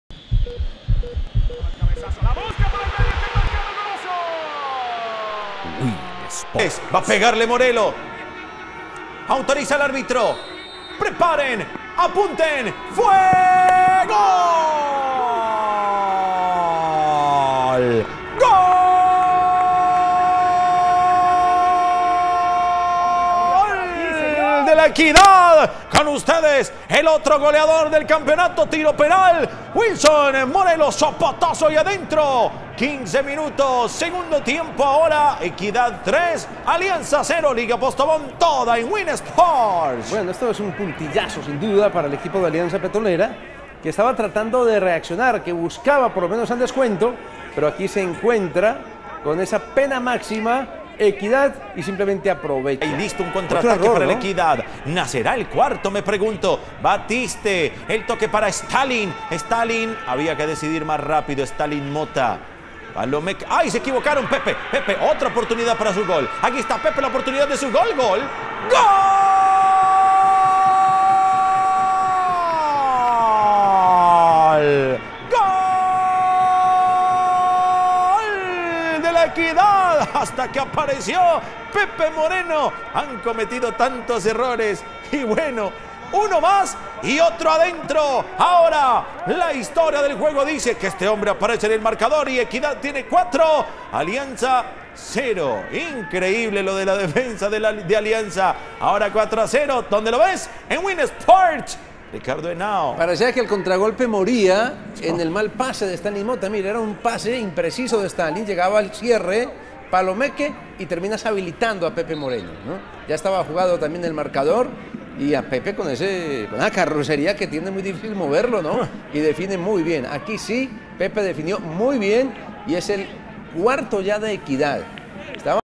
Equidad 4 - 0 Alianza Petrolera - Win Sports
Equidad 4 - 0 Alianza Petrolera
Fecha 9 - Liga Postobón 2013 II